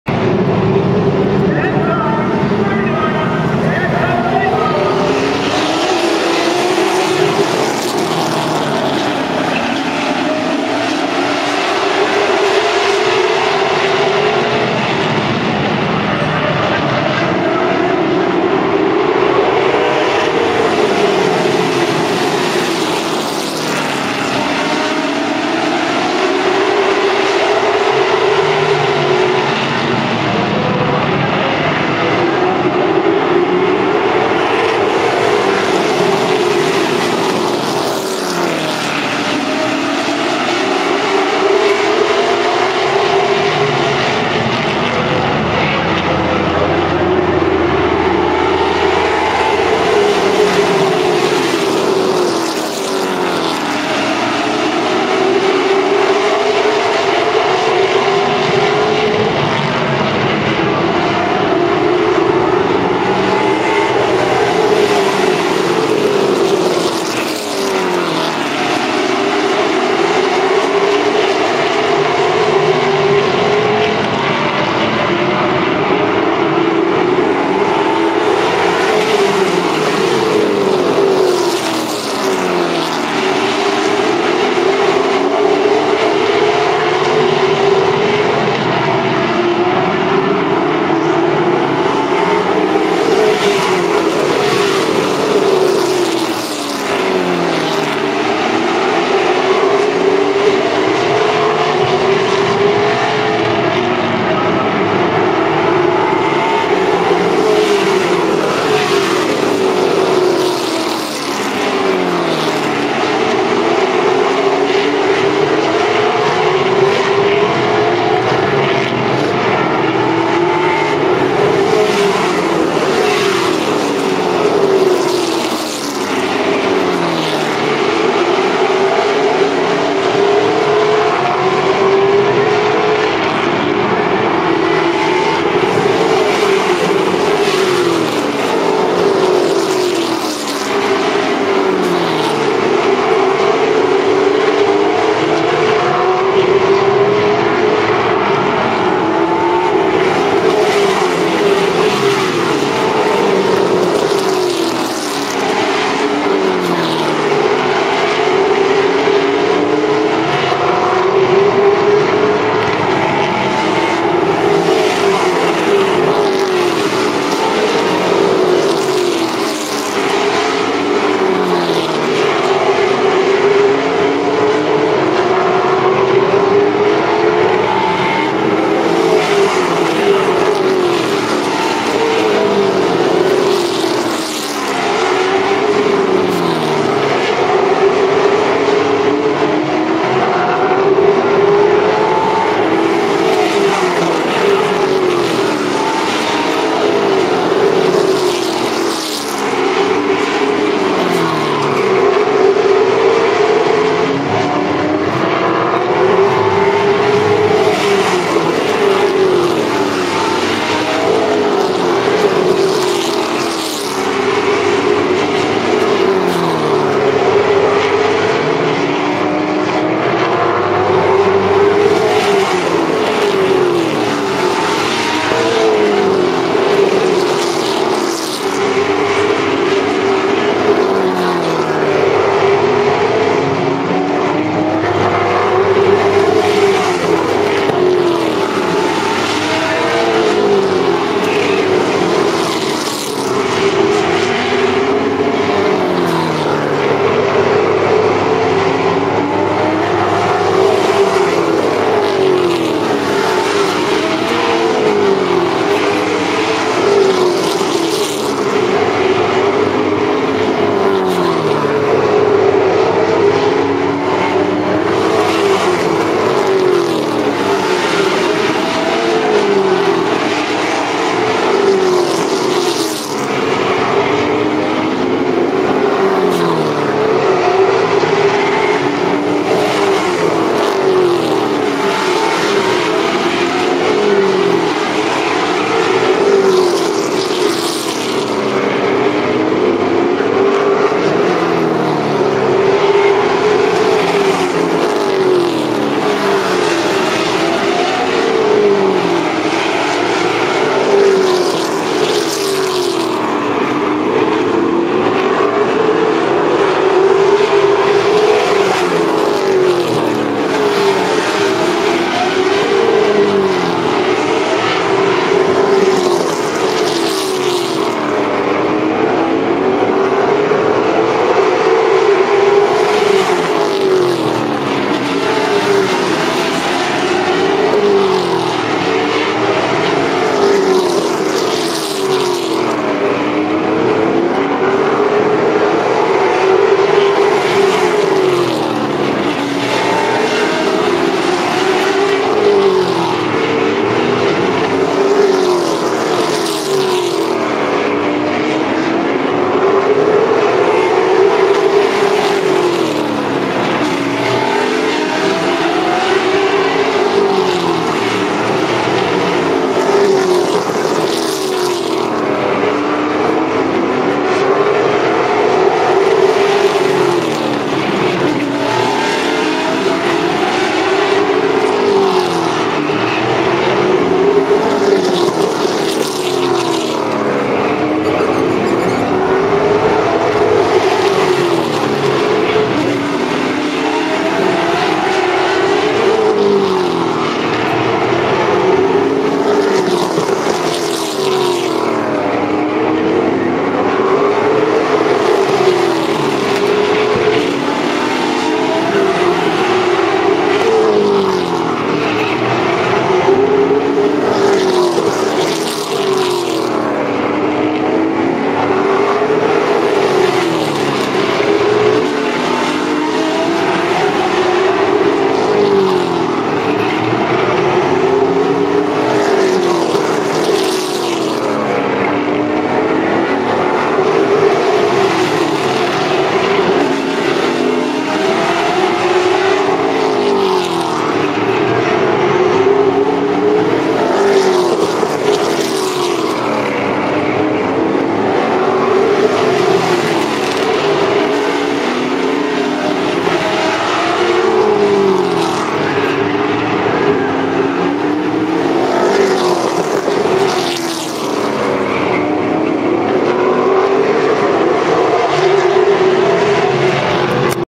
Rockford Speedway Late Model Feature May 10, 2014
39 from the back no yellows